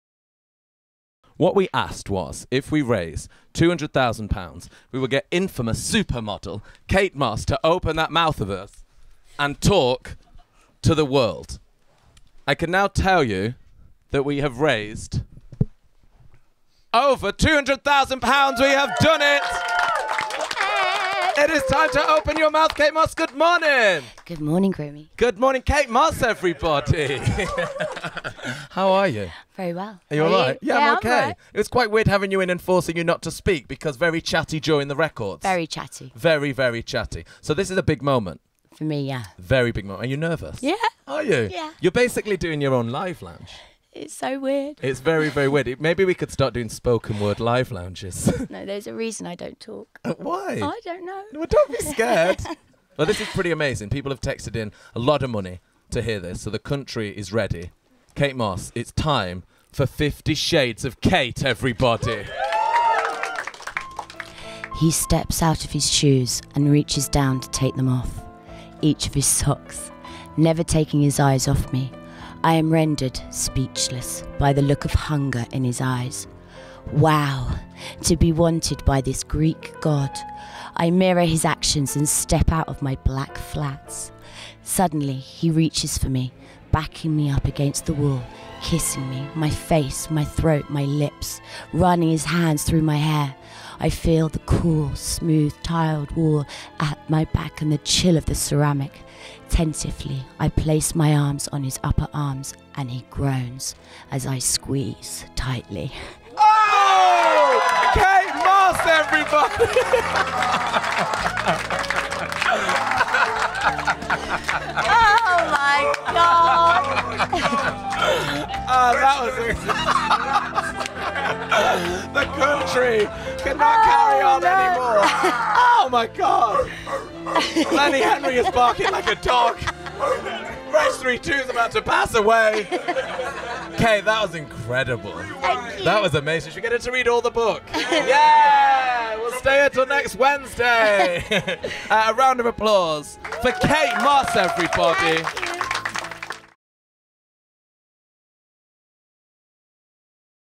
50 Shades Of Kate
Kate Moss reads from 50 Shades of Grey for Comic Relief. See the photos from the Radio 1 Red Nose Challenge